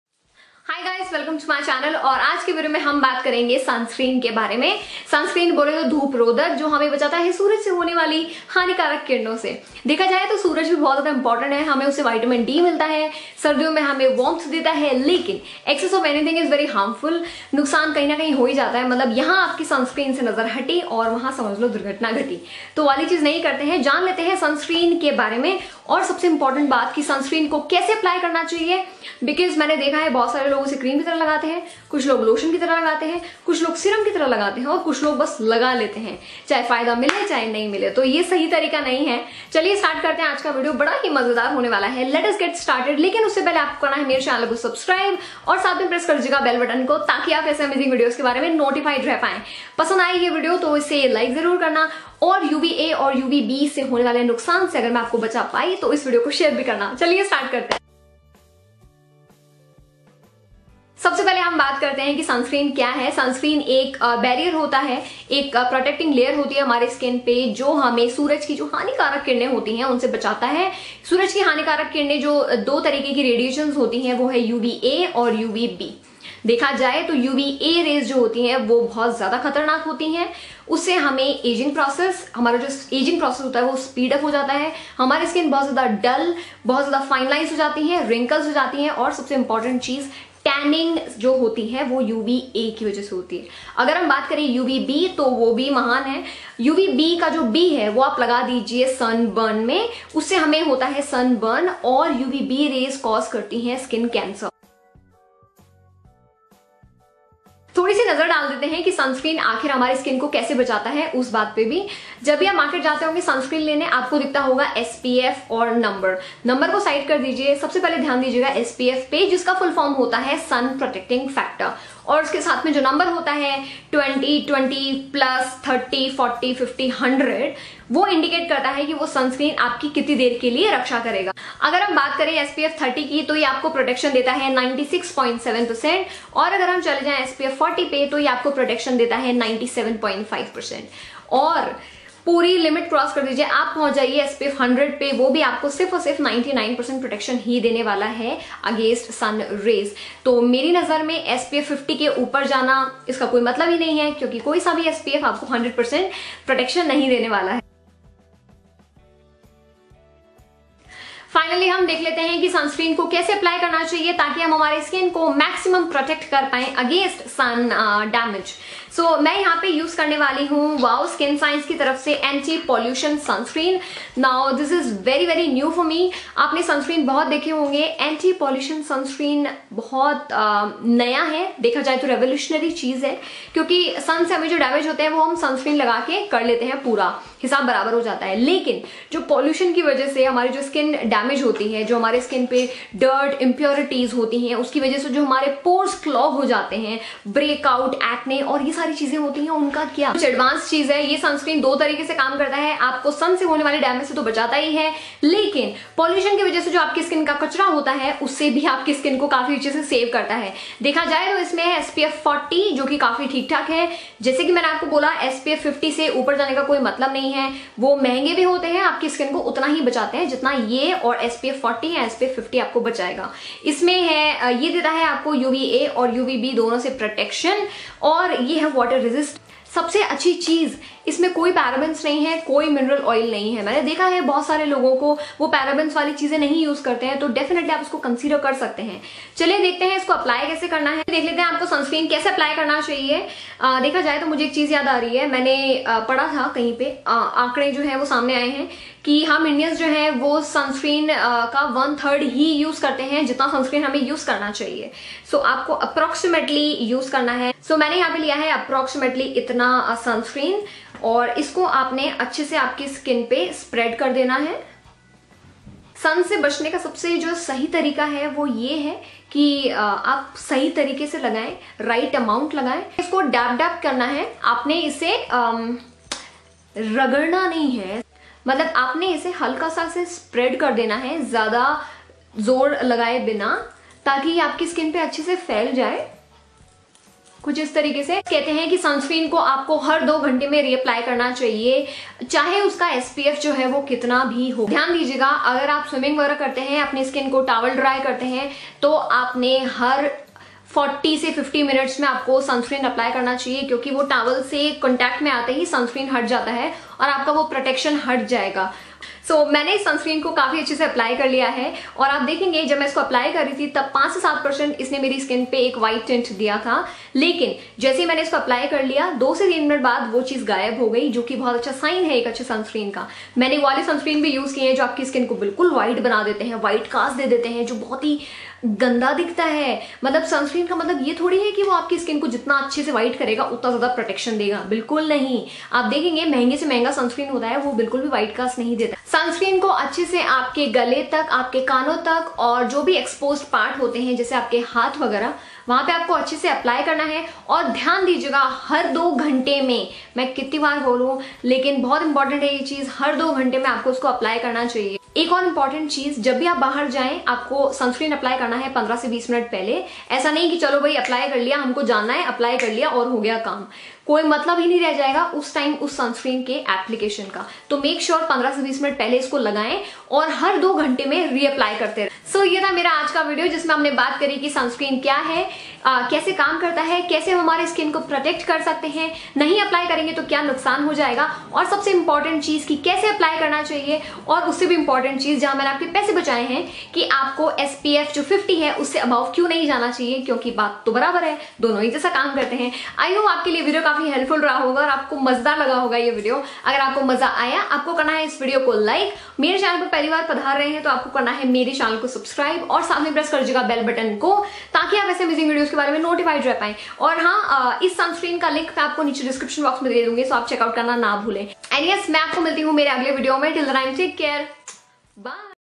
सनस्क्रीन क्या है?? कैसे लगाएं? | How to Apply Sunscreen with Tips & Tricks | Super Style Tips
Sunscreen : WOW Anti Pollution Sunscreen Water Resistant Sunscreen Lotion - No Parabens & Mineral Oils -Shields skin from PM2.5* pollutants

LOVE XOXO love you all 